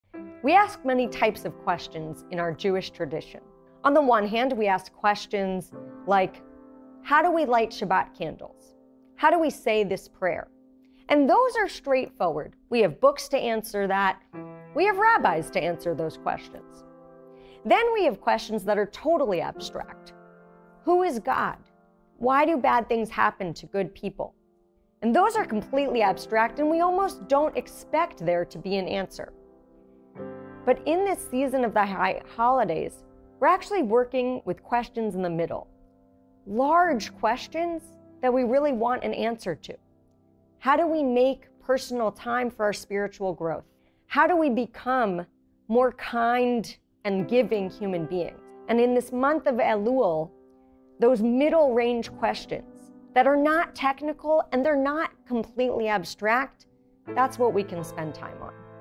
Elul Online Teachings, Day 28 | Rabbi Gavriella Kornsgold
Elul online teachings. Begin your spiritual preparations for the High Holy Days with online lessons from Sinai Temple's clergy for the month of Elul. Day 28, Rabbi Gavriella Kornsgold on asking the right questions